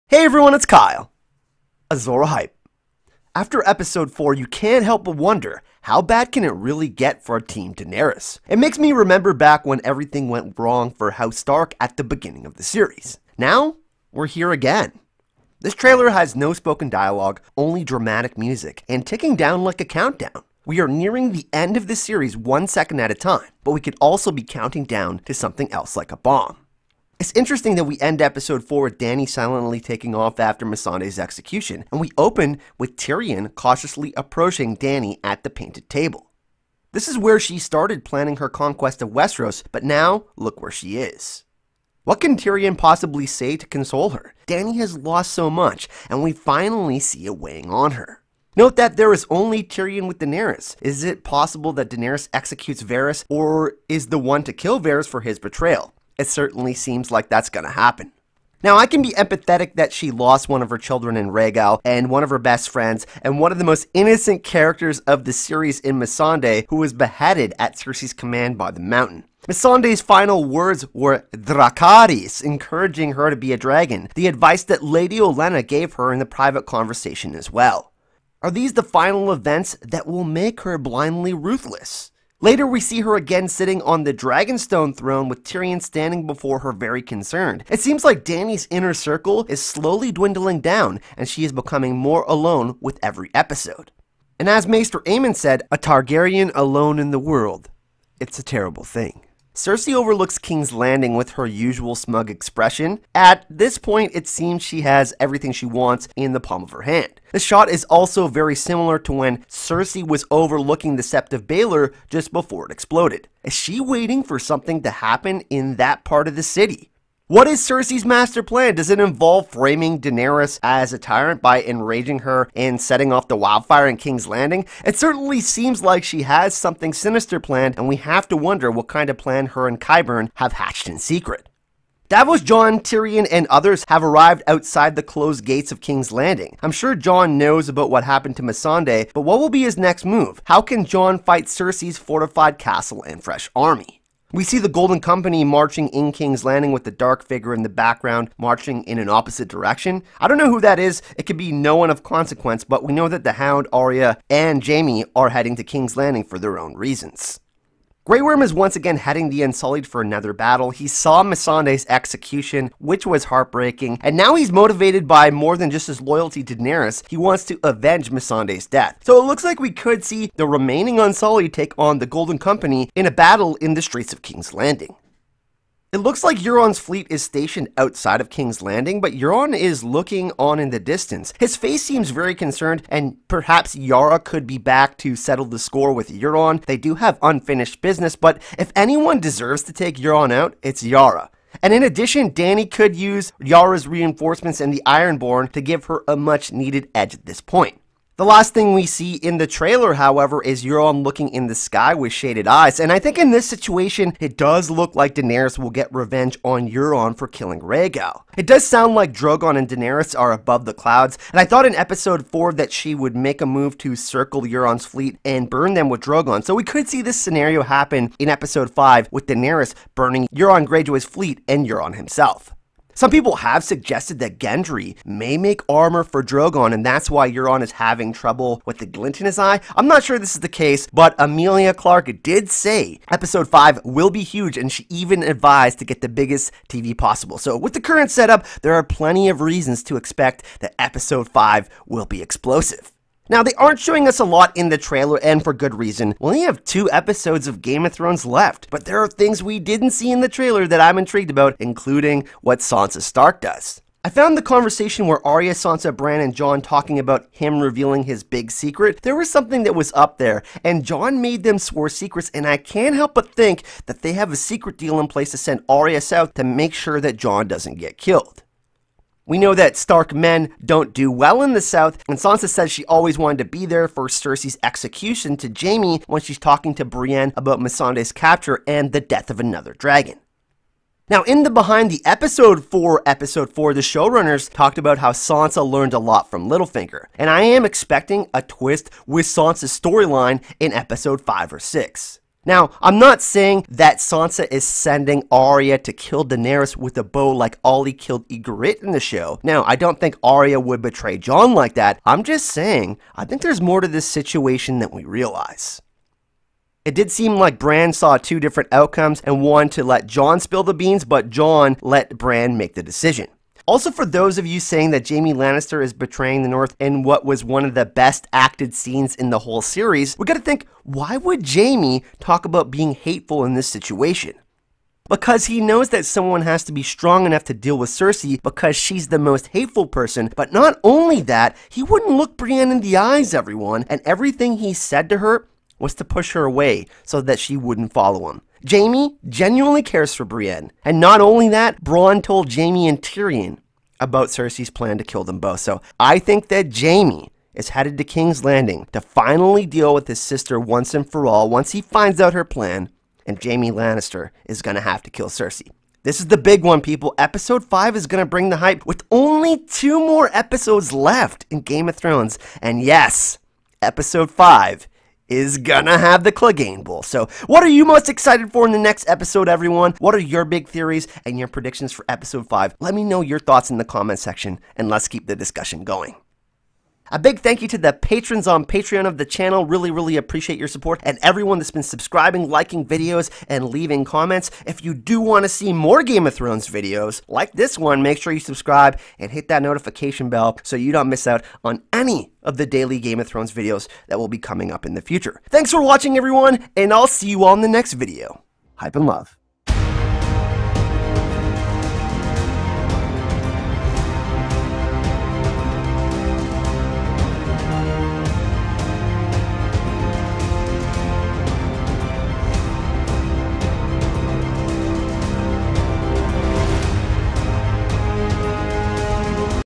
Game of Thrones Season 8 Episode 5 Preview Trailer BREAKDOWN!
Game of Thrones Season 8 Episode 5 is going to be the most intense crazy episode of the Series. In this Video we will Breakdown the Preview Trailer and discuss the End Game for King's Landing, Jon Snow, Cersei Lannister, Daenerys Targaryen, Euron Greyjoy and The Battle for the Iron Throne! There's only 2 more Episodes of Game of Thrones Season 8 left everyone! Let's Get HYPE!

Check out Blaze Manga's Awesome Webstore, and Facebook Page!